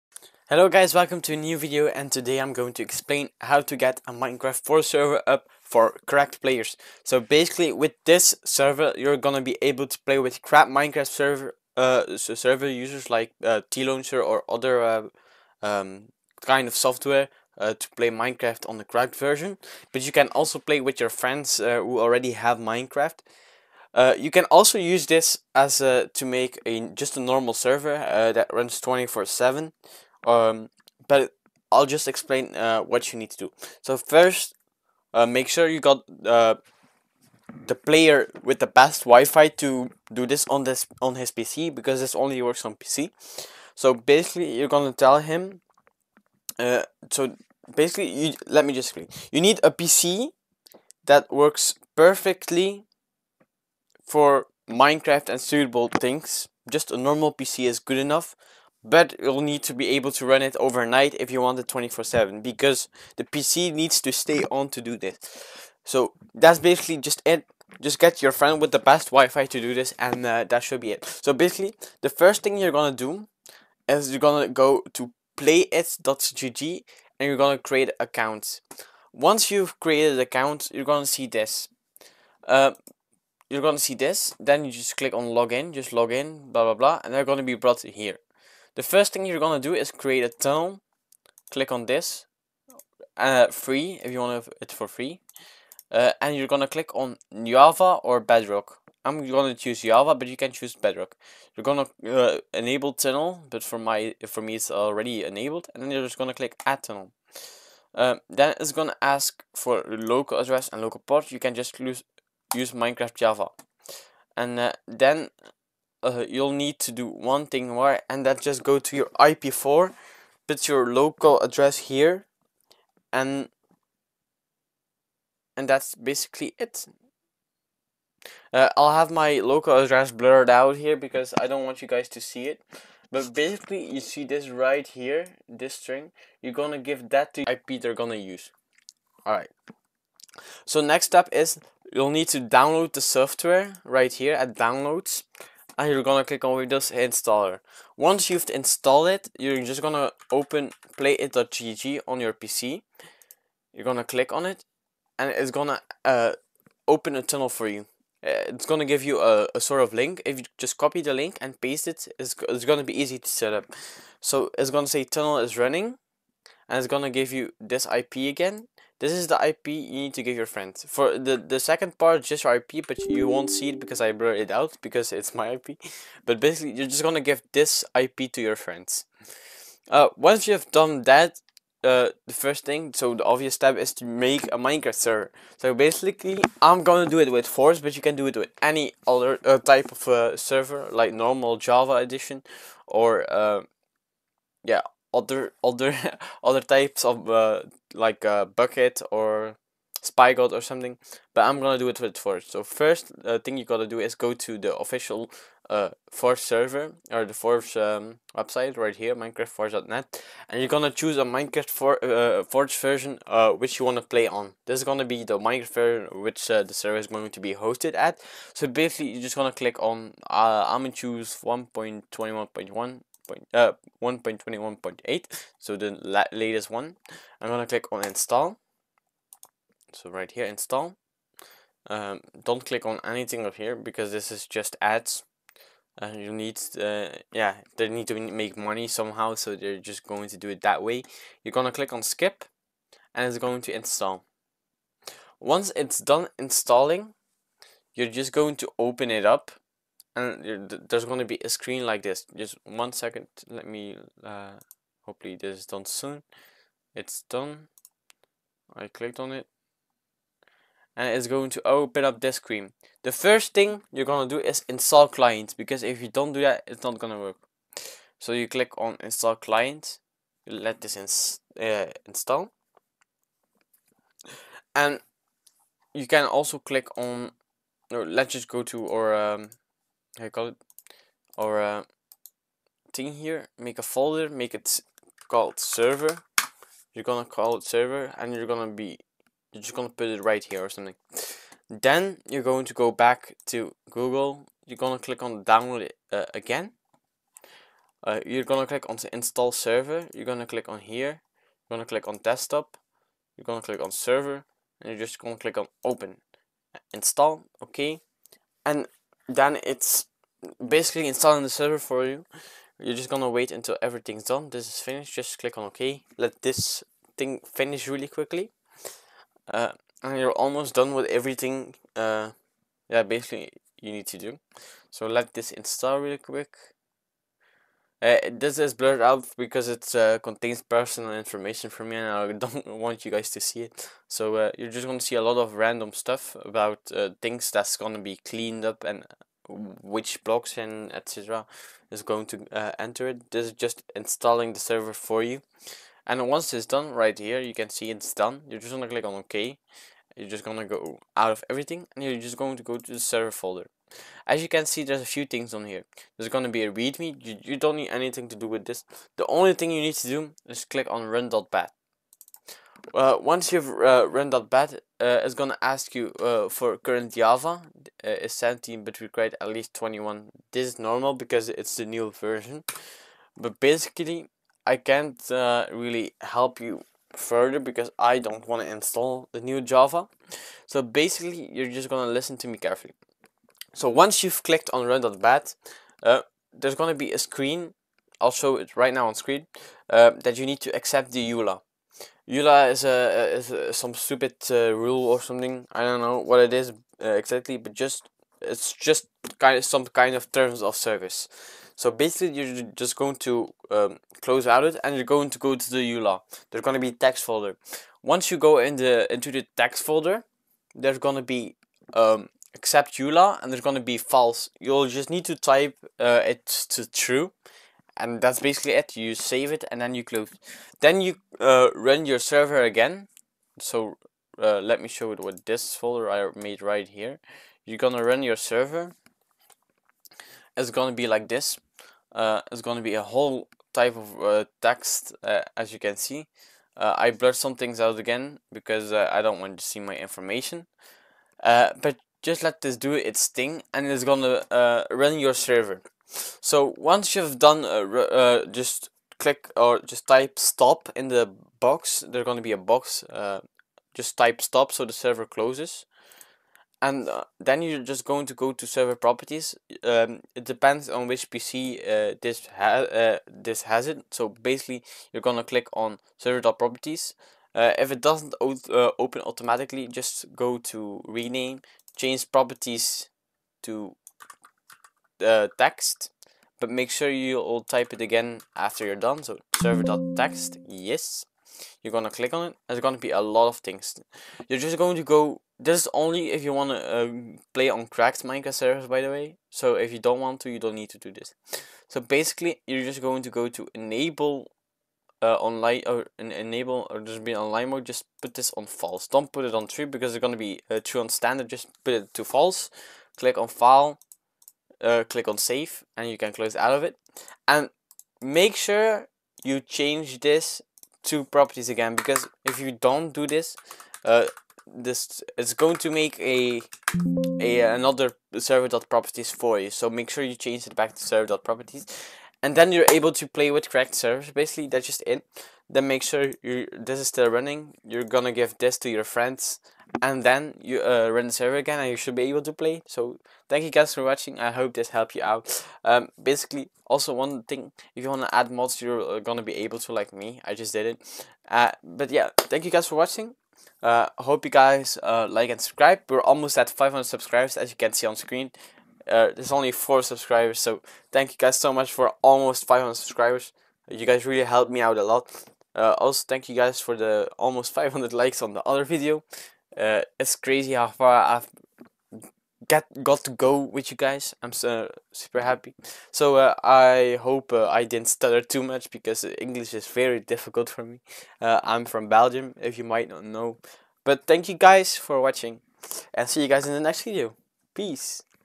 How to make a FREE Server that runs 24/7 Cracked supported! (Step by Step Guide)
Want to host your own server that stays online 24/7 — completely free? 🚀
In this video, I’ll show you how to create a free server you can keep running all the time to play with your friends. You’ll learn how to set everything up, keep it online, and make sure your connection stays stable without paying for expensive hosting plans! 💻

🔹 What’s Included:
✅ Setting up the server software
✅ Making it accessible for friends
✅ Keeping it online 24/7
✅ Tips to improve performance and security

This method is simple, free, and works great for small multiplayer setups!